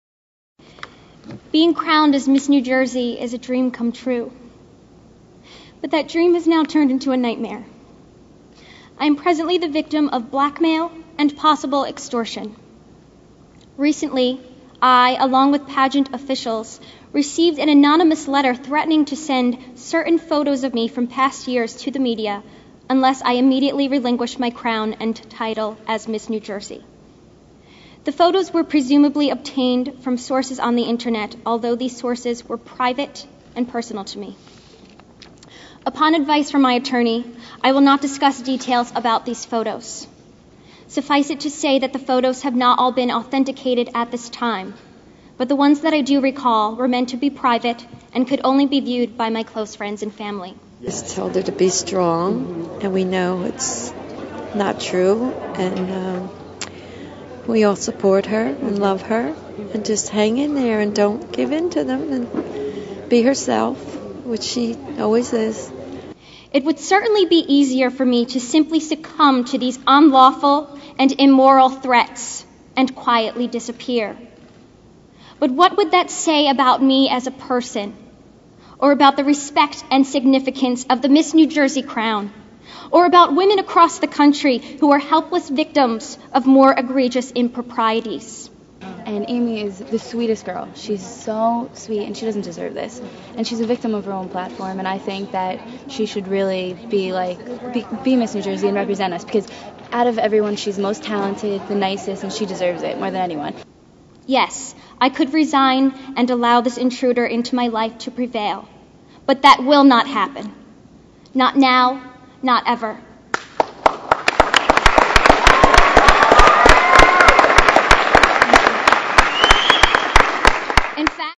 Miss New Jersey Amy Polumbo claims she is being extorted
Miss New Jersey Amy Polumbo announced during a July 12, 2007, new conference in Asbury Park, NJ, that someone was trying to blackmail her with old photographs and demanding that she give up her crown. Video by Thomas P. Costello / Asbury Park Press /